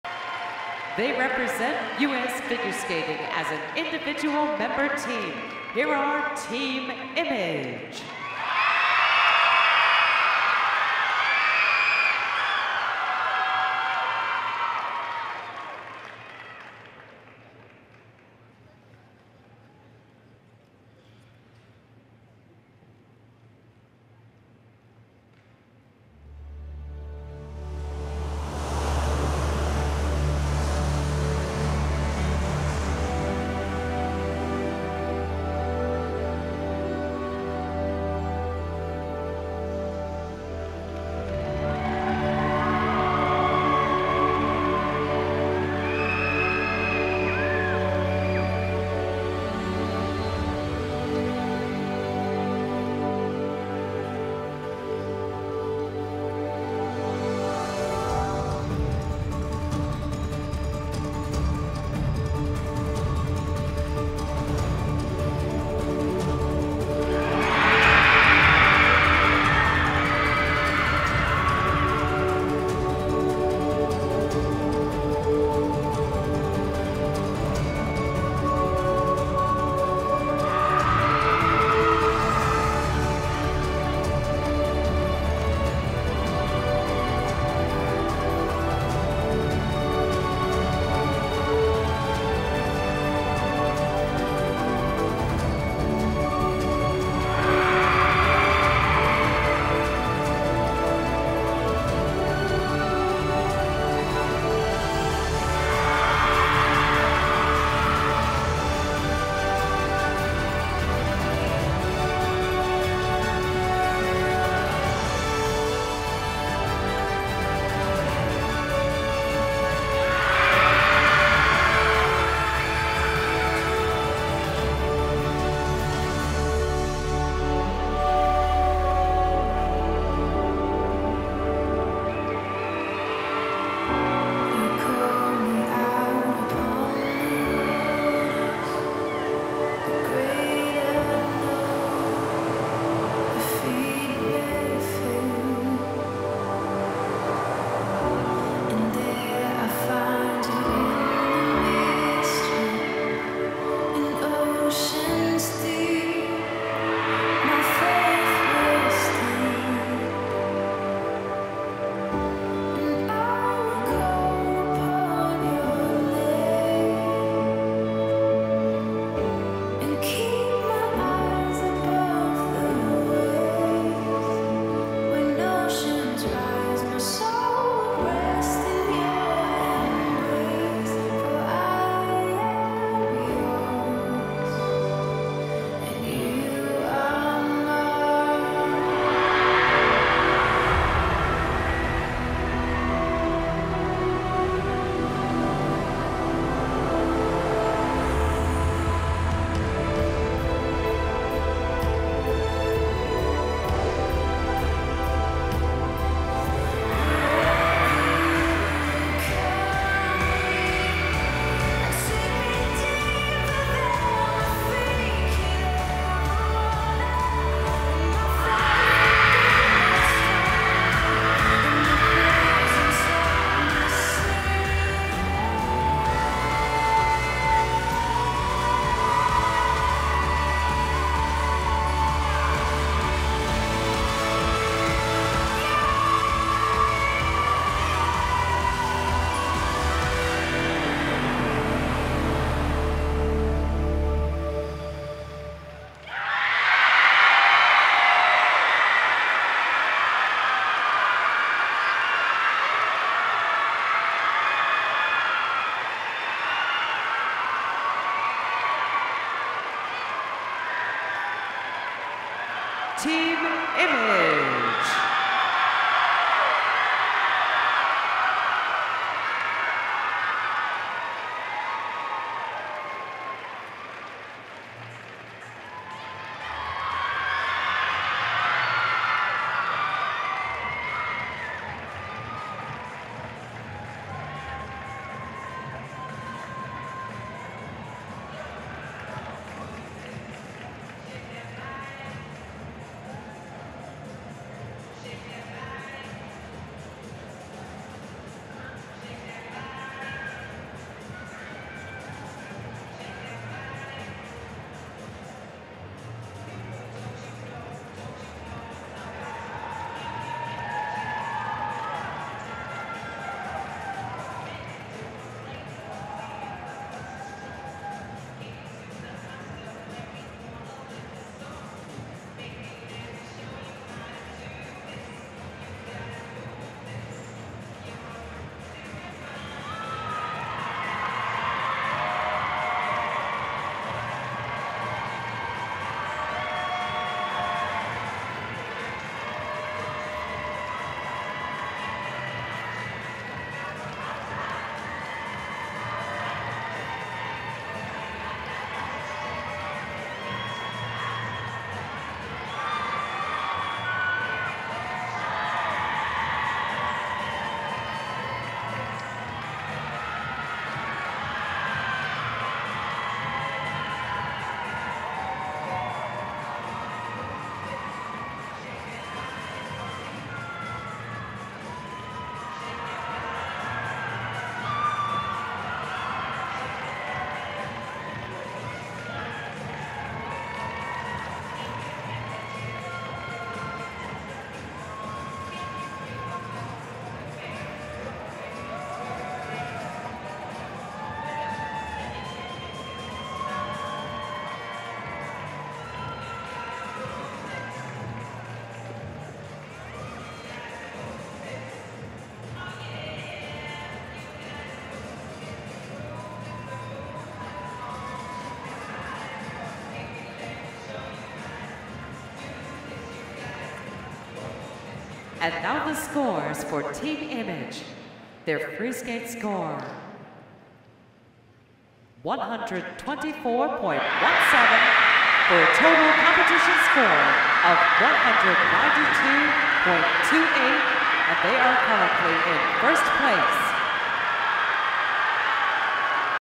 2022 U.S. Synchro Championships Image Junior Free Skate
Team Image Junior’s first-place free program at the 2022 U.S. Synchronized Skating Championships in Colorado Springs, CO.